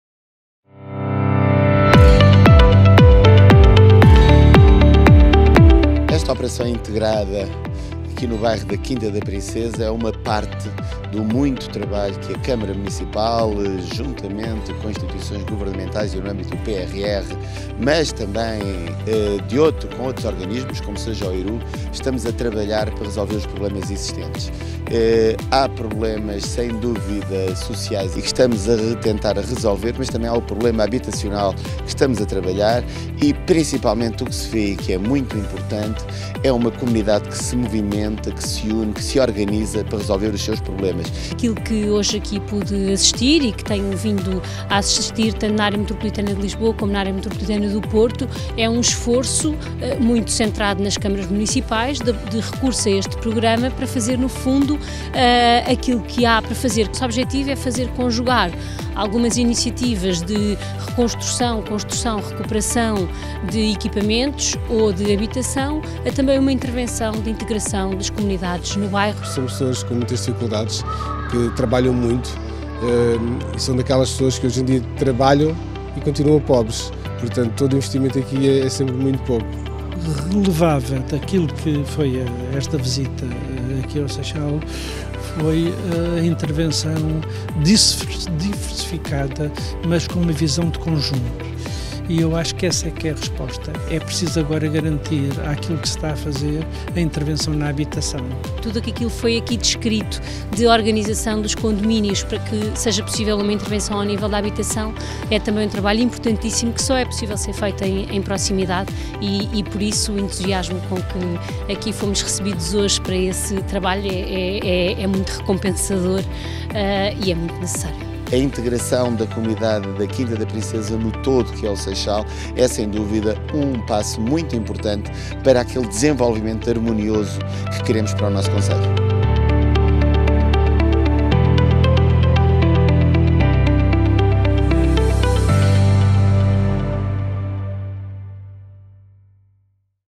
Visita projetos PRR - Comunidades em Ação – Operação Integrada Local da Freguesia de Amora
O trabalho desenvolvido em proximidade pelo Poder Local e diversas instituições parceiras no Bairro da Quinta da Princesa mereceu um investimento de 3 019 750 euros no âmbito do PRR – Comunidades em Ação – Operação Integrada Local da Freguesia de Amora.

Esta terça-feira, a ministra da Presidência e o primeiro-secretário da Área Metropolitana de Lisboa foram recebidos no bairro por Paulo Silva, presidente da Câmara Municipal do Seixal, para contactar com a população e conhecer de perto alguns dos projetos e as instituições que os dinamizam.

A Operação Integrada Local da Freguesia de Amora tem enquadramento na componente Operações Integradas em Comunidades Desfavorecidas nas Áreas Metropolitanas de Lisboa e do Porto do Plano de Recuperação e Resiliência e incide sobre 16 projetos materiais e imateriais, compreendendo iniciativas destinadas à inclusão, integração e cidadania, alfabetização, literacia digital, aprendizagem e ensino, combate ao abandono escolar, promoção da saúde, combate à violência, desporto, valorização cultural e comunitária e promoção da leitura.